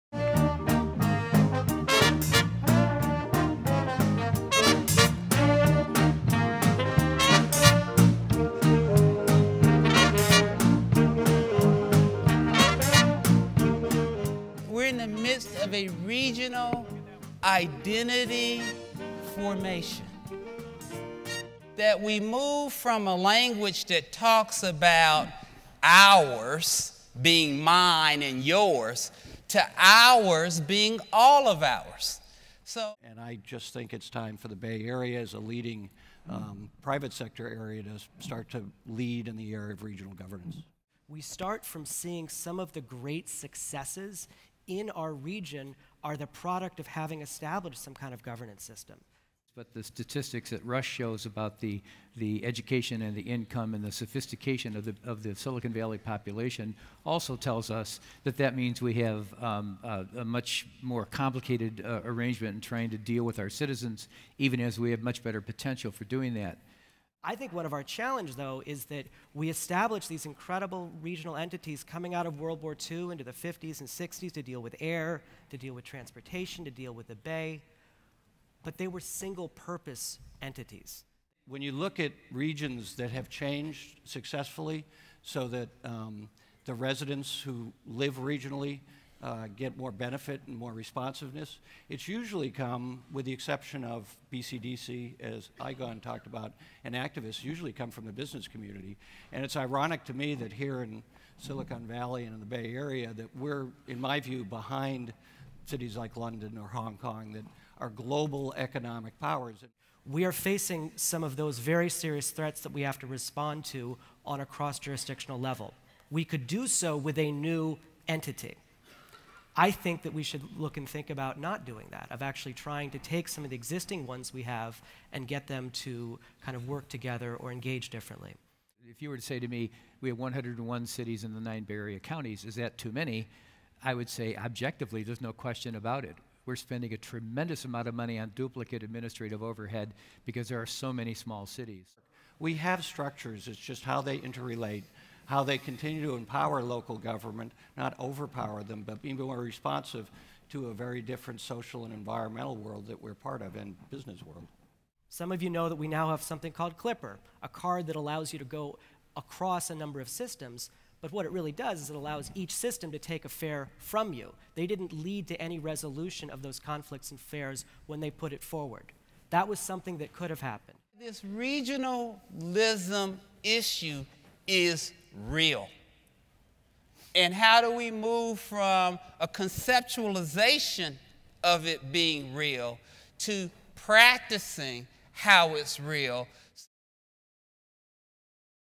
2013 State of the Valley Conference - "Regionalism" Panel Highlights
Panelists:

The Honorable Mark DeSaulnier, California State Senate
Egon Terplan, San Francisco Planning & Urban Research Association
Michael Garvey, City Manager, City of San Carlos (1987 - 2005)
Emmett D. Carson, Ph.D., Silicon Valley Community Foundation (moderator)